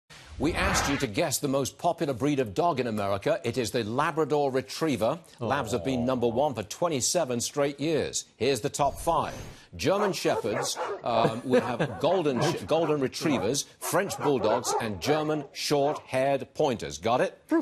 Labs top most popular dog breed list
FBN's Stuart Varney on the top five most popular dog breeds in America.